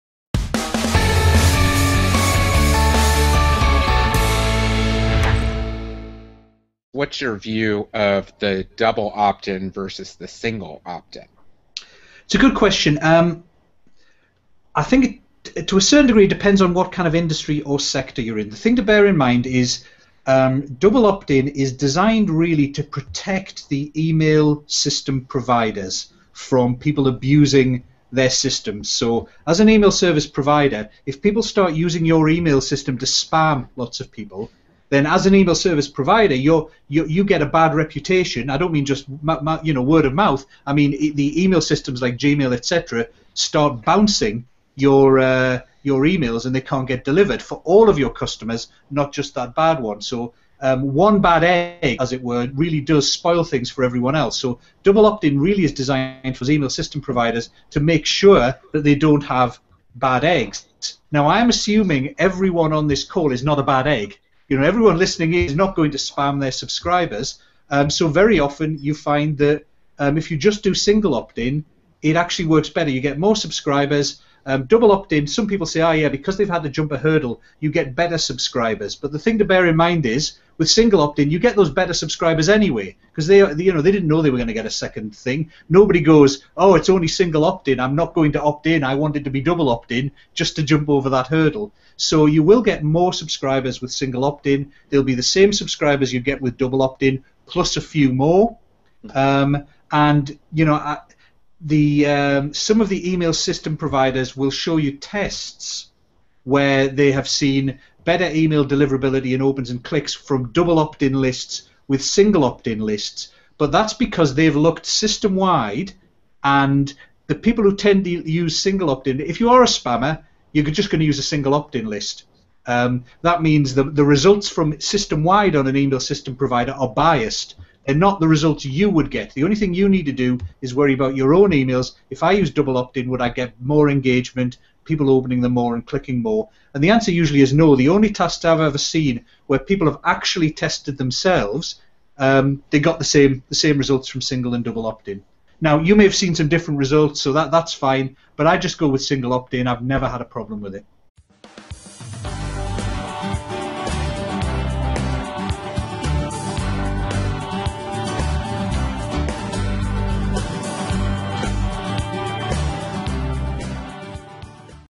What's the difference between the double opt in and the single opt in?
Ian Brodie discusses the differences of Double Opt-In & Single Opt-In.